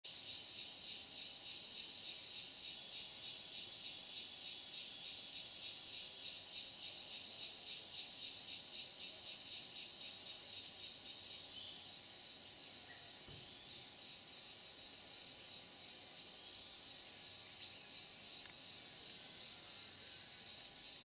Ant nest, Fraser's Hill, Malaysia. 20151031_132534.m2ts
Arboreal ant nest, disturbed montane forest, Fraser’s Hill, Pahang, Malaysia.